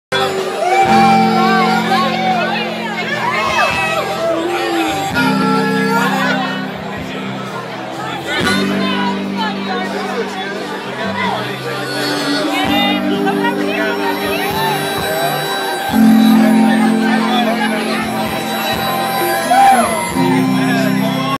JJ grey
Jj grey and mofro at Seville in Pensacola. Camera does bad job on sound. Show was great.